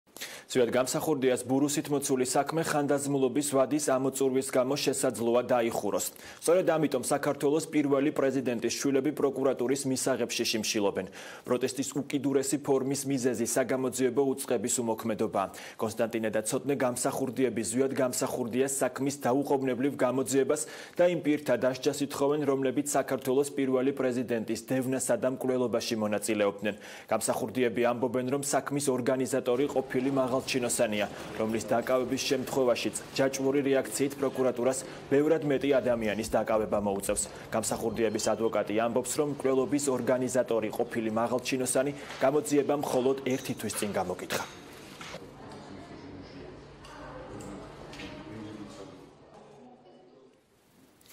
პირველი პრეზიდენტის ოჯახის წევრები პროკურატურასთან შიმშილობენ
ზვიად გამსახურდიას ბურუსითმოცული საქმე ხანდაზმულობის ვადის ამოწურვის გამო, შესაძლოა დაიხუროს. სწორედ ამიტომ, საქართველოს პირველი პრეზიდენტის შვილები პროკურატურის მისაღებში შიმშილობენ. პროტესტის უკიდურესი ფორმის მიზეზი საგამოძიებო უწყების უმოქმედობაა.  კონსტანტინე და ცოტნე გამსახურდიები ზვიად გამსახურდიას საქმის დაუყოვნებლივ გამოძიებას და იმ პირთა დასჯას ითხოვენ, რომლებიც საქართველოს პირველი პრეზიდენტის დევნასა და მკვლელობაში მონაწილეობდნენ.გამსახურდიები ამბობენ, რომ საქმის ორგანიზატორი ყოფილი მაღალჩინოსანია, რომლის დაკავების შემთხვევაშიც ჯაჭვური რეაქციით პროკურატურას ბევრად მეტი ადამიანის დაკავებას მოუწევს. გამსახურდიების ადვოკატი ამბობს, რომ მკვლელობის ორგანიზატორი მაღალჩინოსანი გამოძიებამ მხოლოდ 1 თვის წინ დაკითხა.